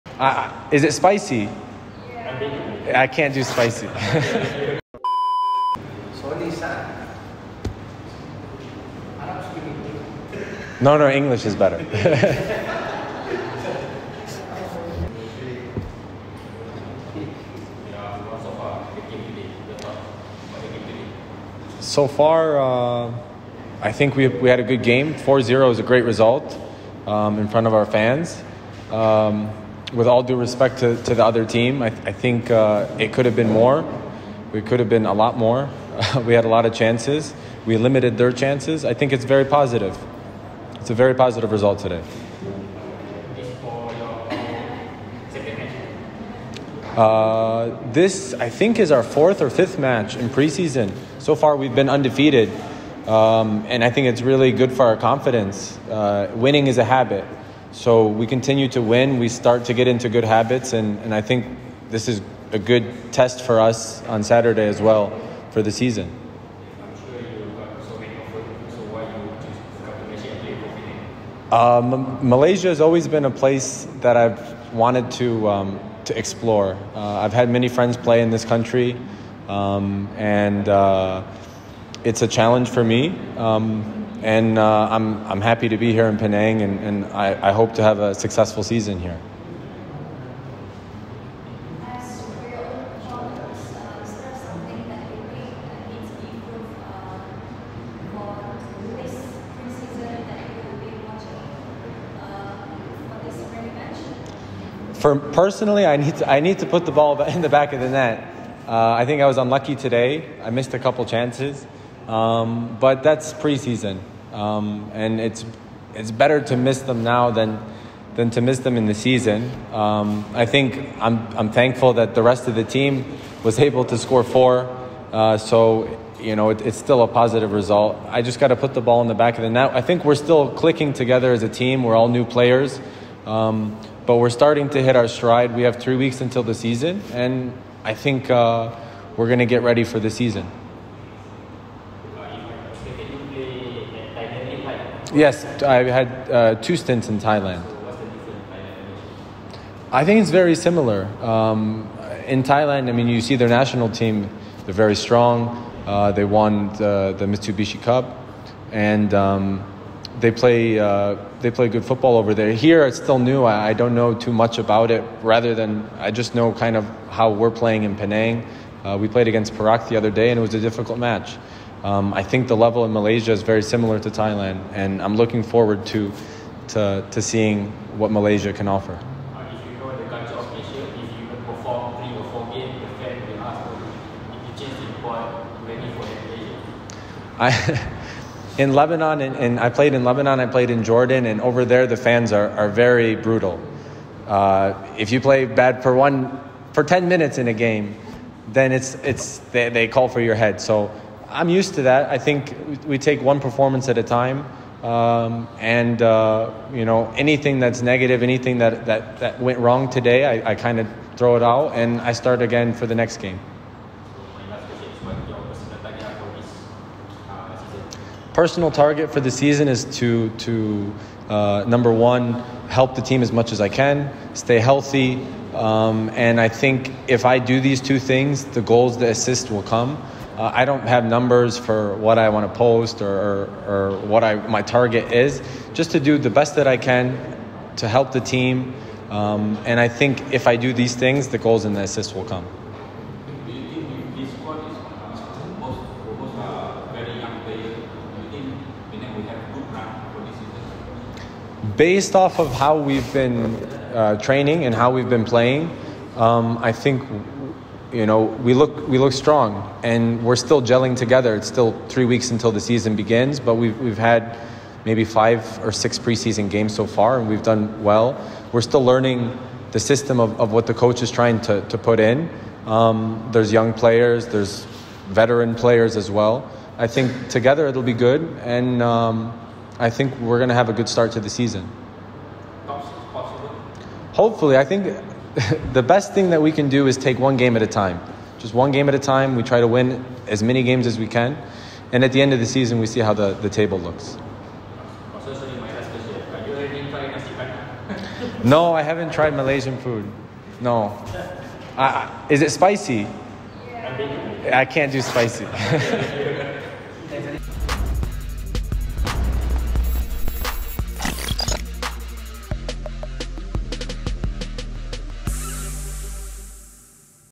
"Saya sudah bersedia hadapi cabaran dan kecaman sebab ketika di Lebanon dan Jordan ia lebih sukar"
Temu bual bersama pemain import baru Penang FC, Soony Saad selepas tamat perlawanan Piala Harapan dengan keputusan 4-0 menentang pasukan dari Singapura, Geylang International FC.

Untuk rekod, Sonny pernah bermain untuk beberapa kelab di liga Thailand termasuk PT Prachuap dan Police Tero FC.